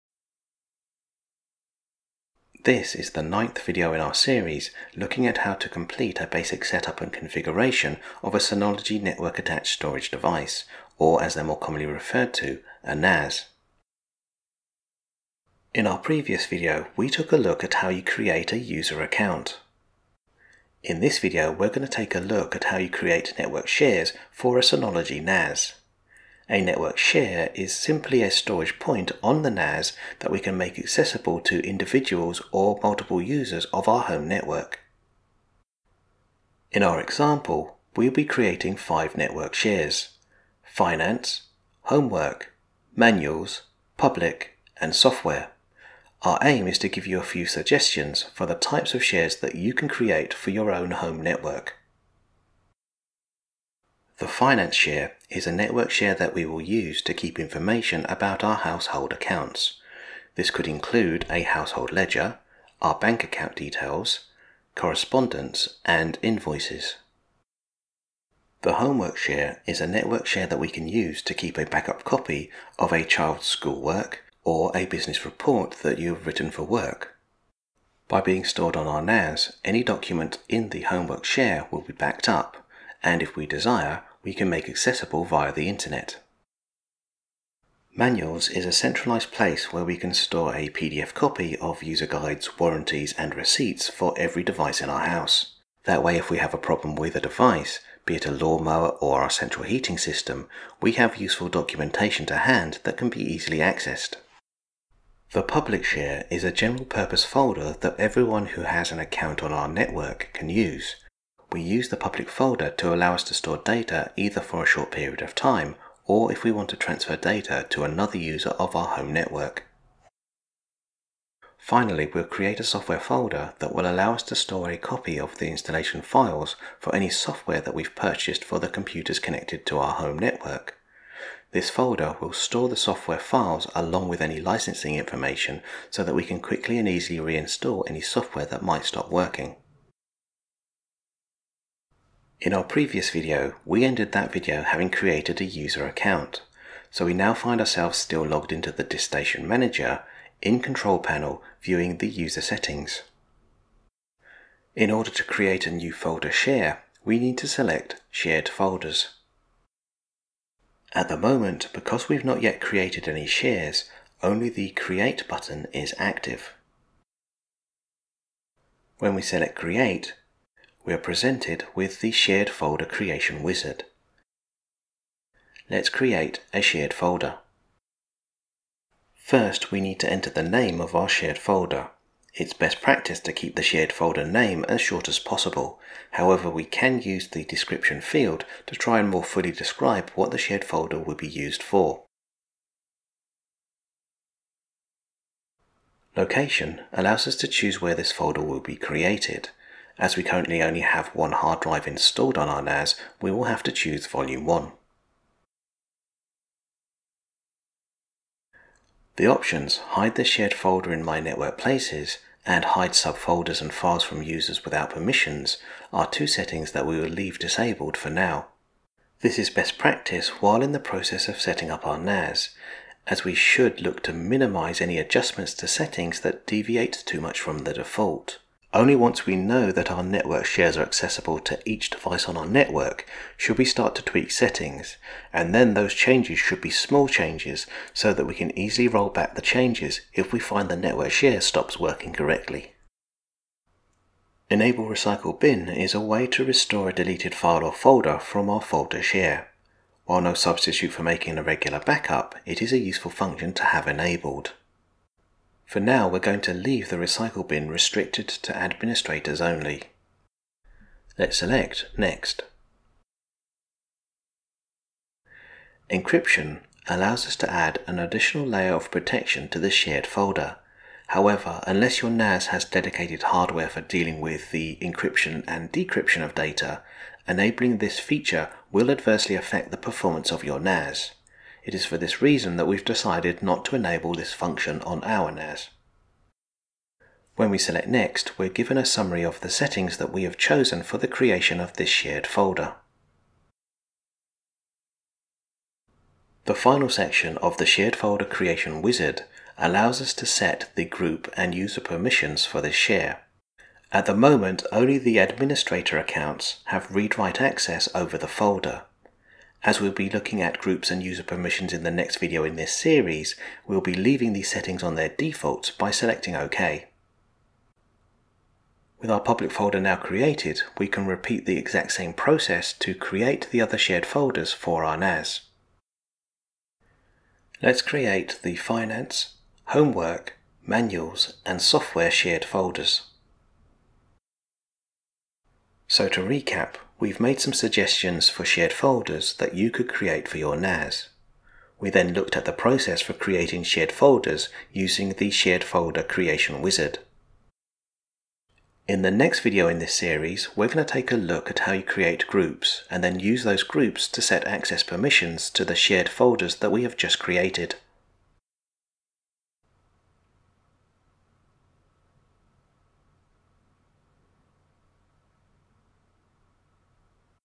How to setup a Synology NAS (DSM 6) - Part 9: Creating Network Folder Shares
In this series of video's we take a look at how you perform the basic setup and configuration of a Synology, Network Attached Storage (NAS) device. The aim of this series of videos is to try and offer a reference point to assist you in the setup of your own NAS.

We start this video by giving you a few suggestion's for the types of  network shares that you would typically create within a home network. We then take a look at the process for creating folder shares on a Synology NAS.

◽My preferred online shop is Amazon

Products seen in this video:

Synology DS218j 2 Bay Desktop NAS
Western Digital Red 2Gb HD
Seagate Iron Wolf 2Gb HD